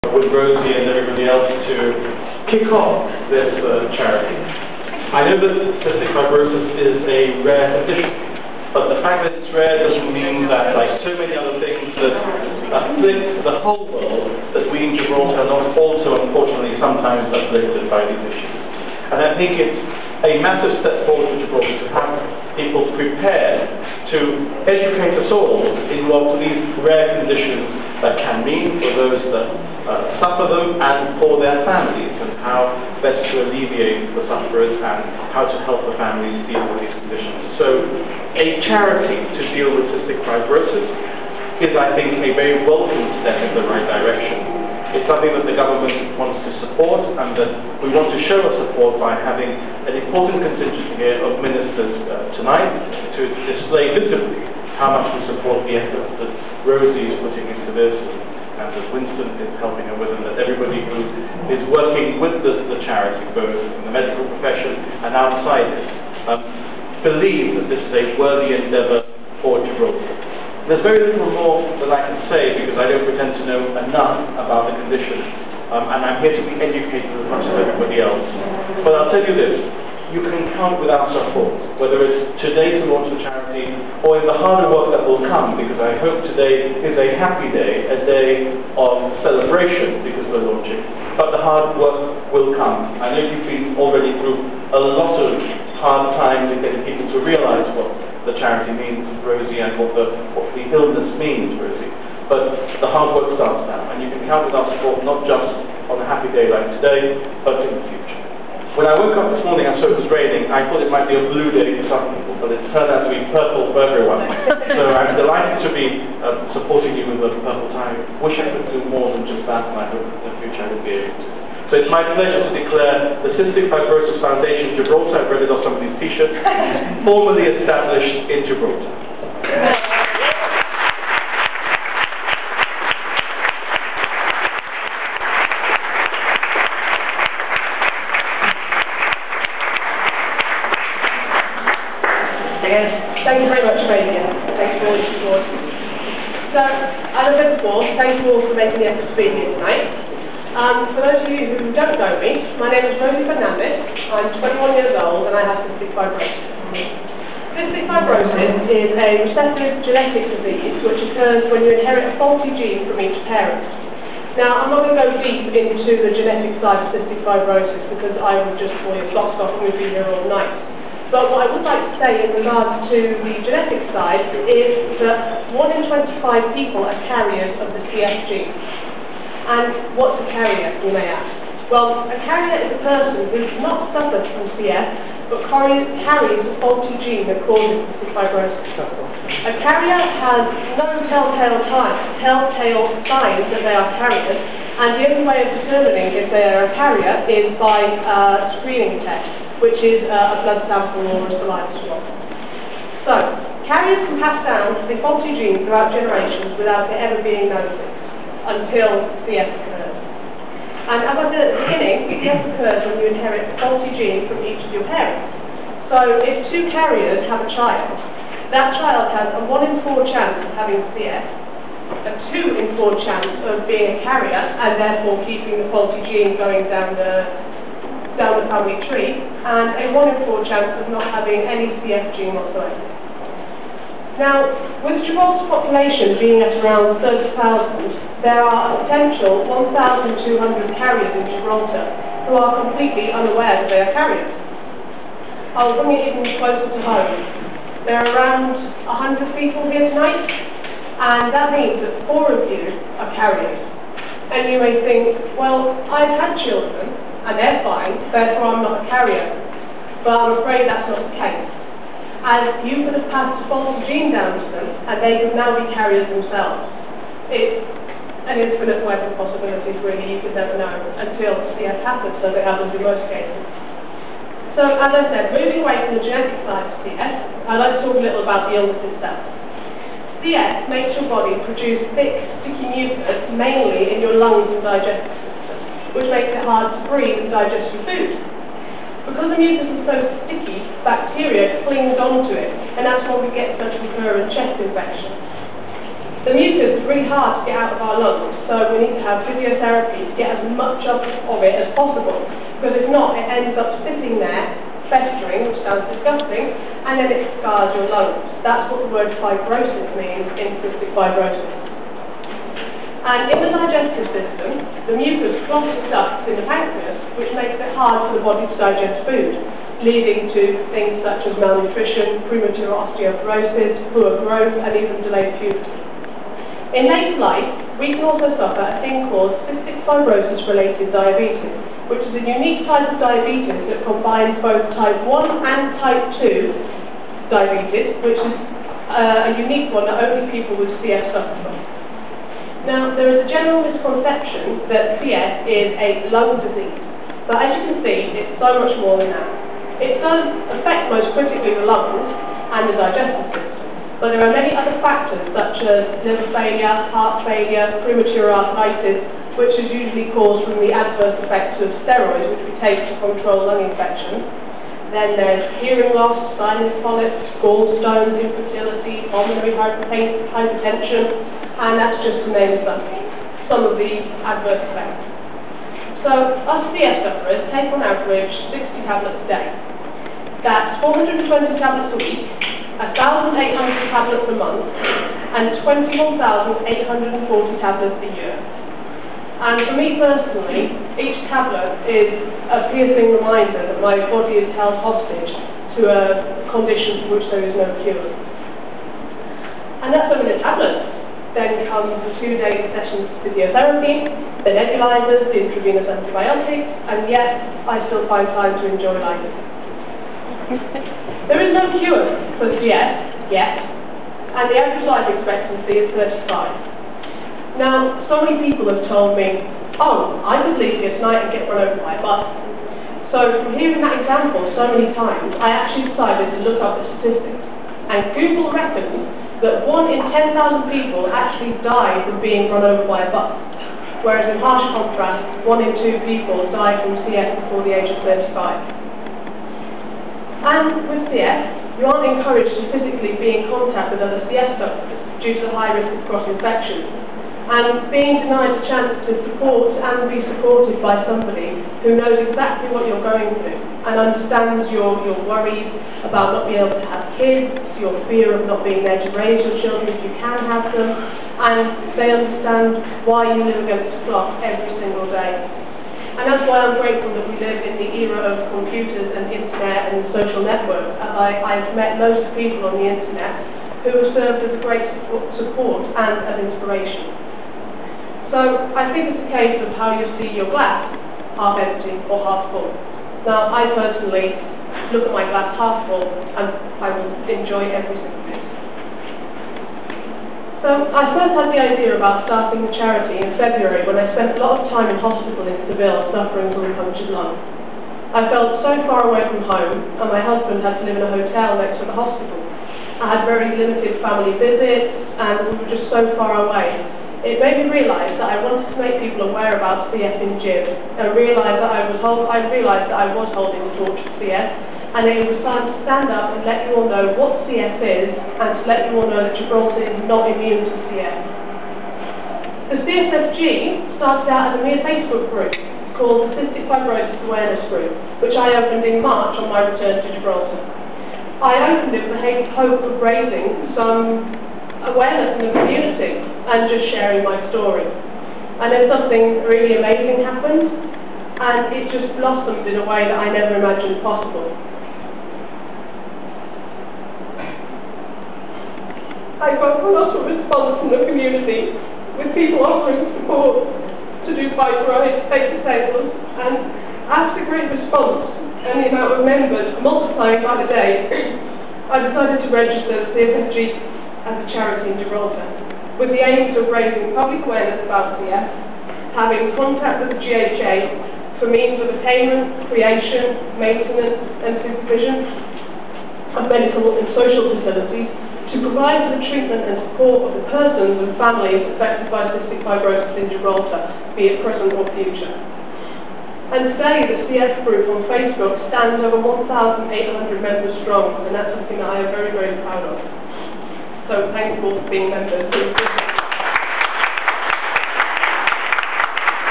Cystic Fibrosis Foundation Gibraltar Launch Party
Rosie Fernandez and Chief Minister Fabian Picardo